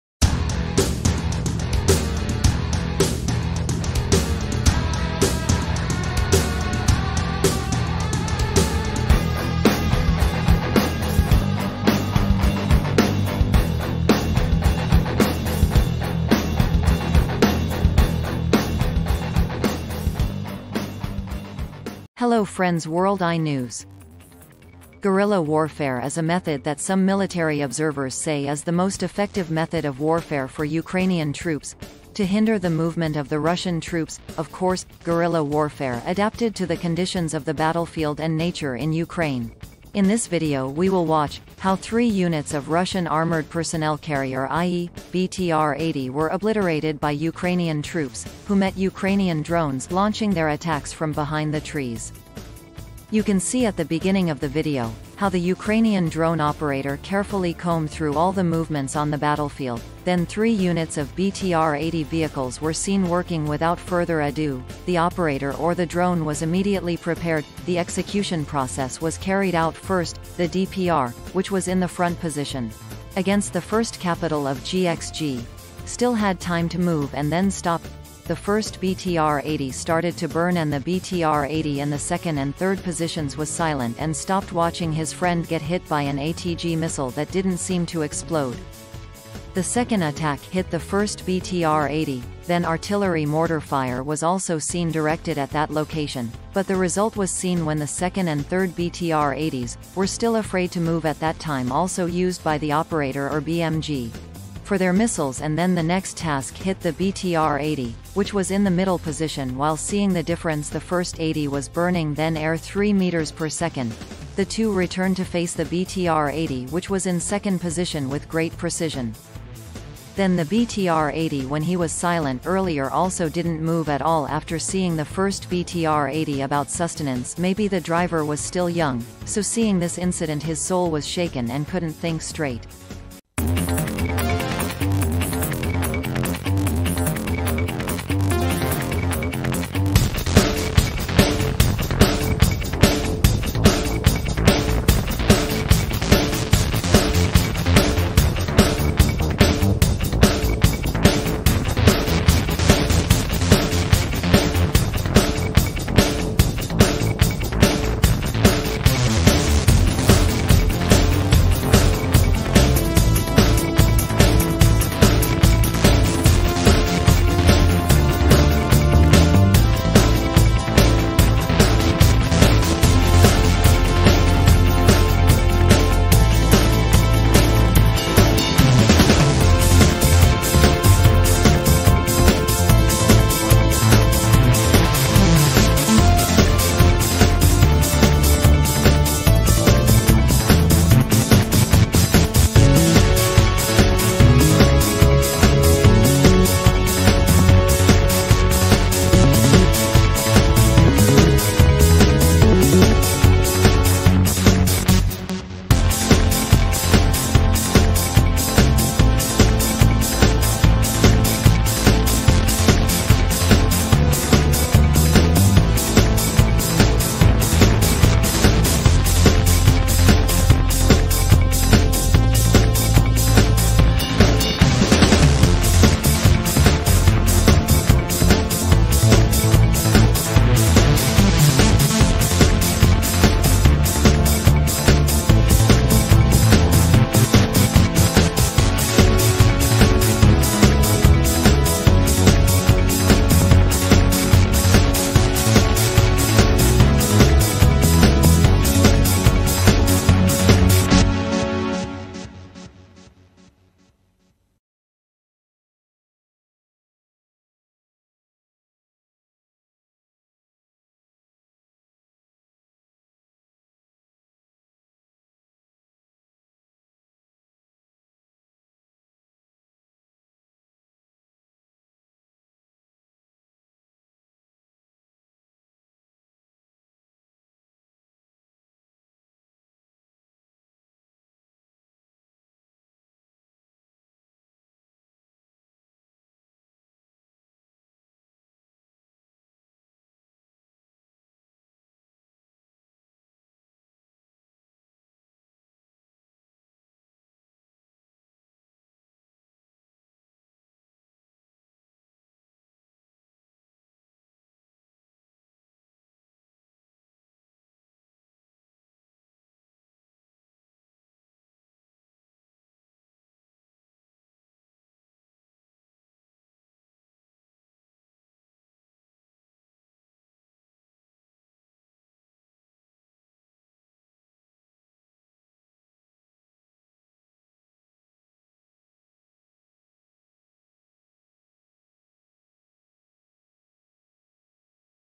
IN 2 MINUTES, 3 RUSSIAN BTR-80 TANKS BECOME SOLD IRON!!!
Guerrilla warfare is a method that some military observers say is the most effective method of warfare for Ukrainian troops. To hinder the movement of the Russian troops, of course, guerrilla warfare adapted to the conditions of the battlefield and nature in Ukraine,
 in this video we will watch. How 3 units of Russian armored personnel carrier i.e. btr-80 were obliterated by Ukrainian troops, who met Ukrainian drones launching their attacks from Behind the trees.
You can see at the beginning of the video how the Ukrainian drone operator carefully combed through all the movements on the battlefield, then 3 units of btr-80 vehicles were seen working without further ado, the operator or the drone was immediately prepared, the execution process was carried out first, the DPR, which was in the front position .
 against the first capital of gx G. Still had time to move and then stopped. The first btr-80 started to burn and the btr-80 in the second and third positions was silent and stopped watching his friend get hit by an ATG missile that didn't seem to explode.